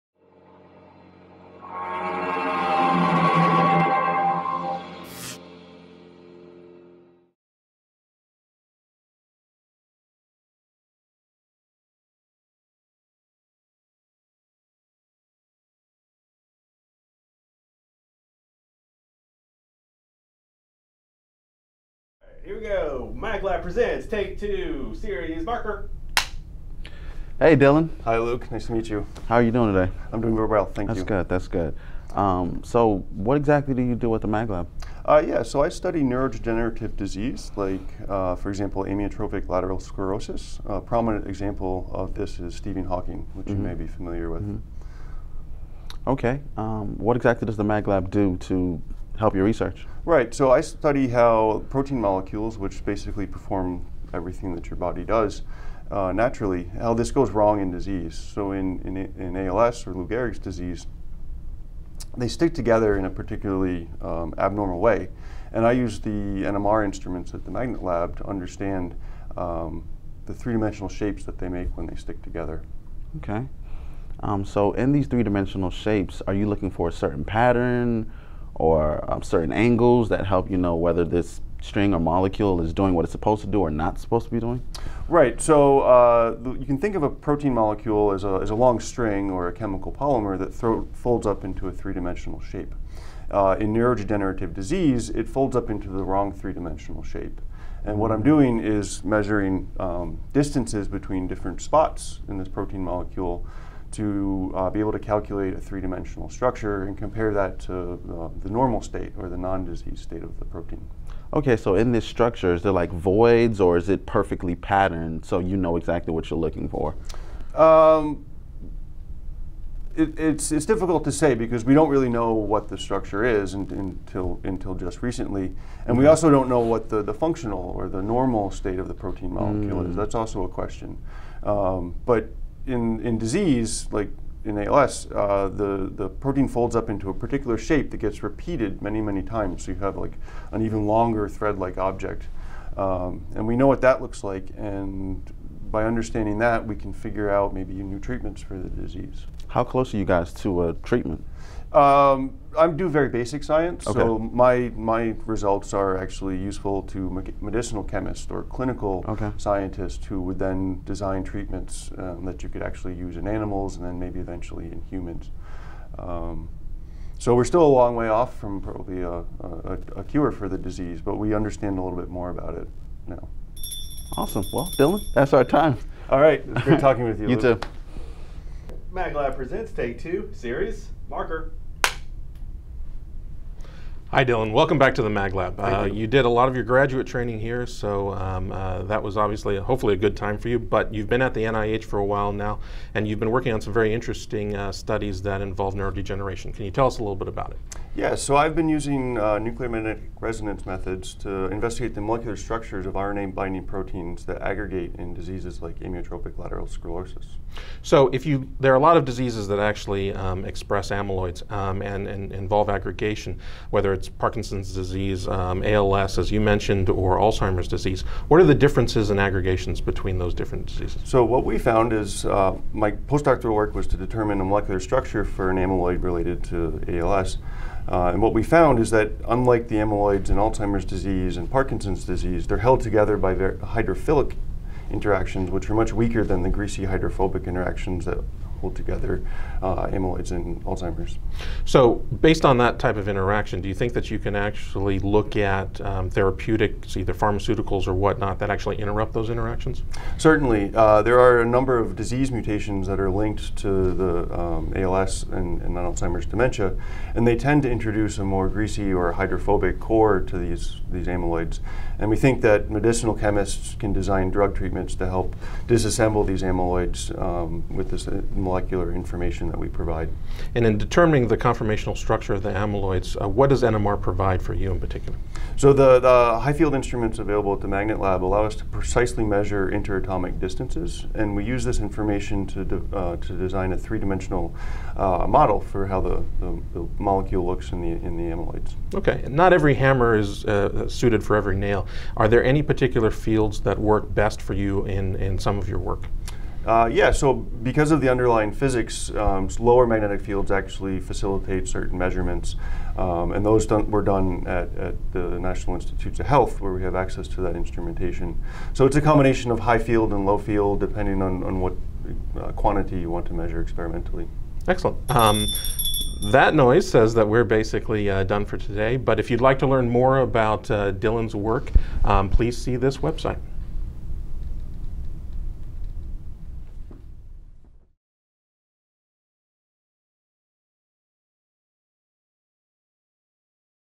Take 2 for Science: Dylan Murray
Biophysicist Dylan Murray explains his research in high magnetic fields in two takes: first to a non-scientist, then (in more technical terms) to a fellow scientist.